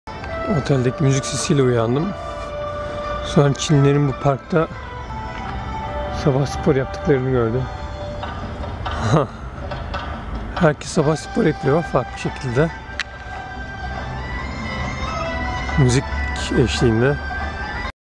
Çin'de bir sabah
Urumçi'nin Çin mahallesinde bir otelde uyurken kulağıma bir müzik sesi çalındı. Pencereden baktım, güneş henüz yeni doğmak üzere ve parkta bir sürü Çinli toplu halde spor yapıyor. Makinemi kaptığım gibi sokağa fırladım.